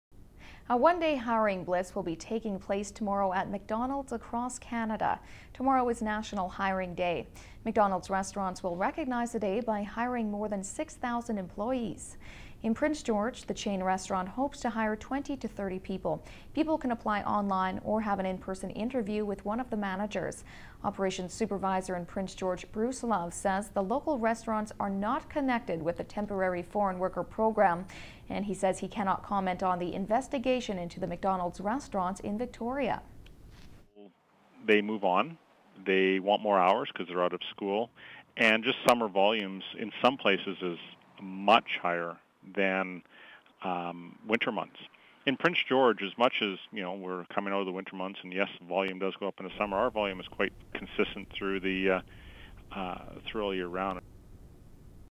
McDonald's Holding Hiring Blitz Across Canada
A one day hiring blitz will be happening tomorrow at McDonald's across Canada. Tomorrow is National Hiring Day. McDonald's restaurants will recognize the day by hiring more than 6,000 employees.  In Prince George, the chain restaurant hopes to hire 20-30 people. People can apply online or have an in person interview with one of the managers. Operations Supervisor in Prince George, Bruce Love, says the local restaurants are not connected with the Temporary Foreign Worker Program and he cannot comment on the investigation into the McDonald's restaurants in Victoria.